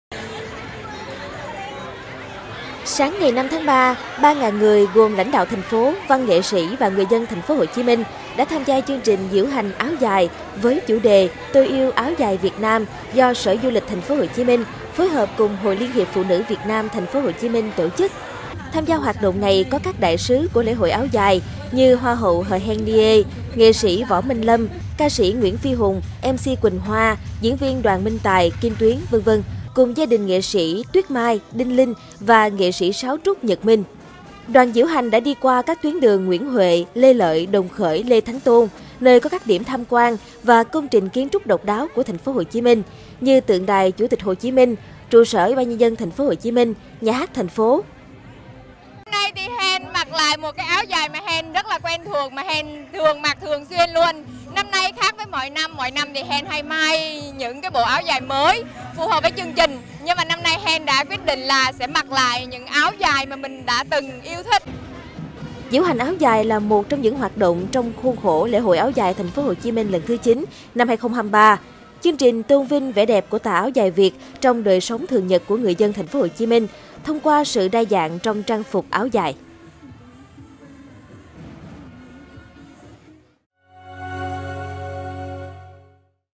3.000 người diễu hành áo dài ở TP HCM
Sáng 5-3, 3.000 người gồm lãnh đạo thành phố, văn nghệ sĩ và người dân TP.HCM đã tham gia chương trình diễu hành áo dài với chủ đề 'Tôi yêu áo dài Việt Nam'.